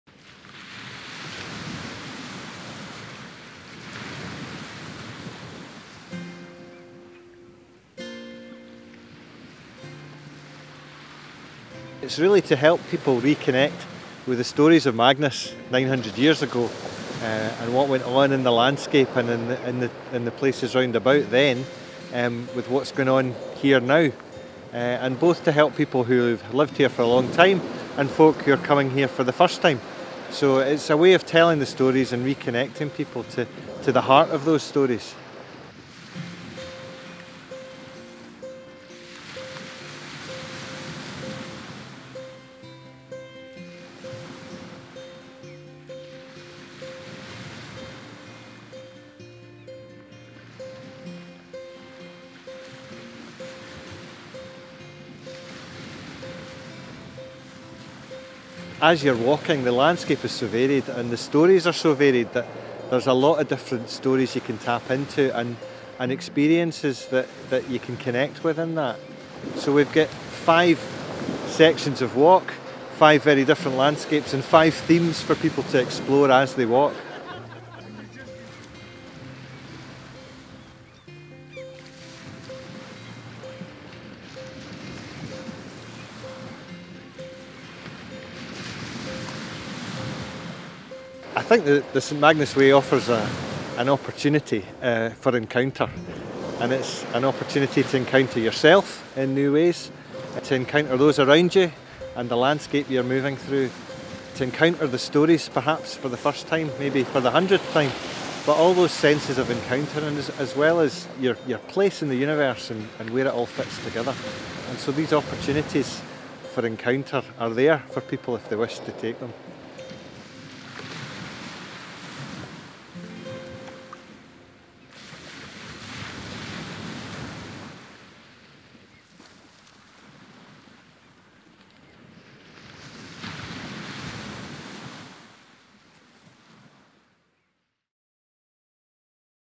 St Magnus Way launched in Orkney
A new long distance walking route, taking in some of Orkney's most spectacular scenery, has been launched. The St Magnus Way is a 55 mile pilgrimage trail designed to mark the 900th anniversary of the martyrdom of St Magnus. Find out more from the official

Thanks to David McNeish for the soundtrack.